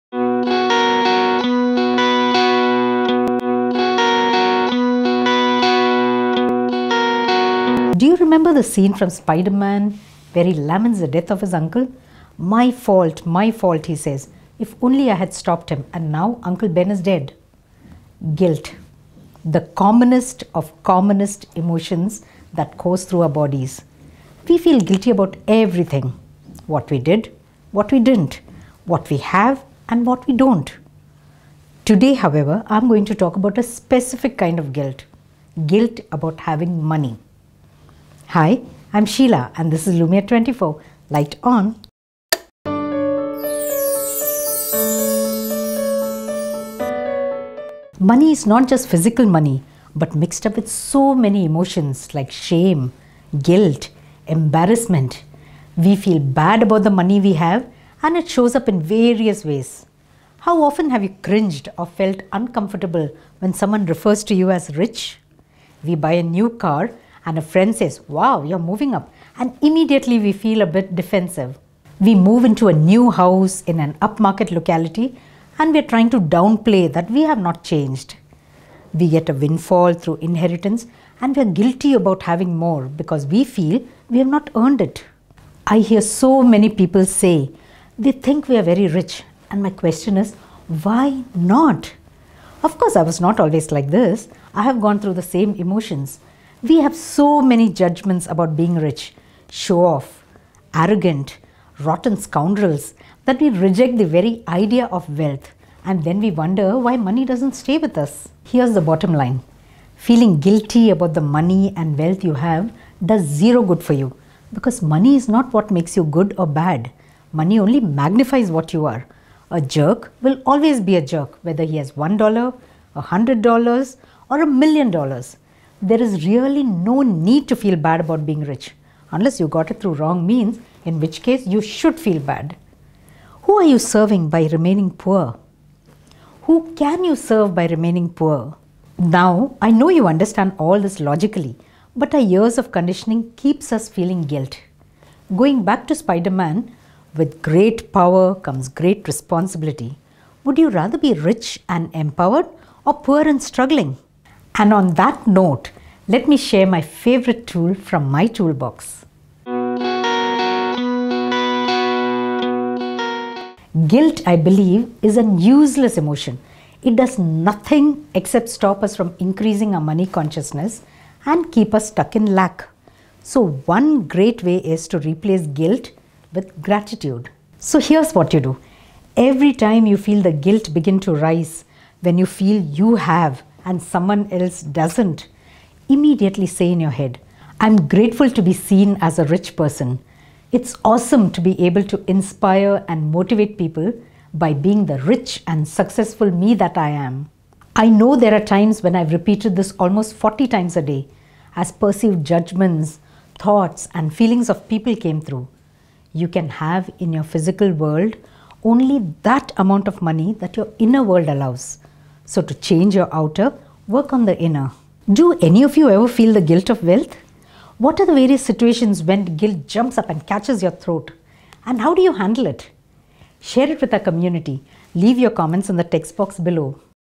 Here's What's Choking Your Money Flow | Sheila Ram Mohan | Best Life Hacks | Lumiere 24
One thing that you can do to increase your money. Do you remember the scene from Spiderman where he laments the death of his uncle…my fault, my fault, he says…if only I had stopped him…and now uncle ben is dead.  Guilt…the commonest of the commonest emotions that course through our bodies…we feel guilty about everything…what we did , what we didn’t… what we have and what we don’t. Today, however, I am going to talk about a specific kind of guilt….guilt about having money.

Money is not just physical money but mixed up with so many emotions like shame, guilt, embarrassment…we feel bad about the money we have…and it shows up in various ways. How often have you cringed or felt uncomfortable when someone refers to you as rich?  We buy a new car and a friend says ‘ wow you are moving up’ & immediately we feel a bit defensive
We move in to a new house in an upmarket locality and we are trying to downplay that we have not changed…
We get a windfall , through inheritance and we are guilty for having more, because we feel we have not earned it.
I hear so many people saying “ They think we are very rich”…and my question is WHY NOT
Of course , I was not always like this, I have gone through the same emotions…We have so many judgements about  being rich , “ show –off” ‘arrogant’, “rotten scoundrels’ that we reject the very idea of wealth…and then we wonder why money doesn’t stay with us!
Here’s the bottom line! Feeling guilty about the money and wealth you have does zero good for you. Because money is not what makes you good or bad. ..money only magnifies what you are.
….a jerk will always be a jerk whether he has one dollar, a hundred dollars or million dollars.
There’s really no need to feel bad about being rich( unless you got it through wrong means-in which case you should feel bad)
Who are you serving by remaining poor? Who CAN you serve by remaining poor?
Now I know you understand all this logically, but our years of conditioning keep us feeling guilt.
Now going back to Spiderman, “with great power comes great responsibility” …would you rather be rich and empowered or poor and struggling?
And on that note, let me share my favourite tool from my tool box
Sheila’s tool box.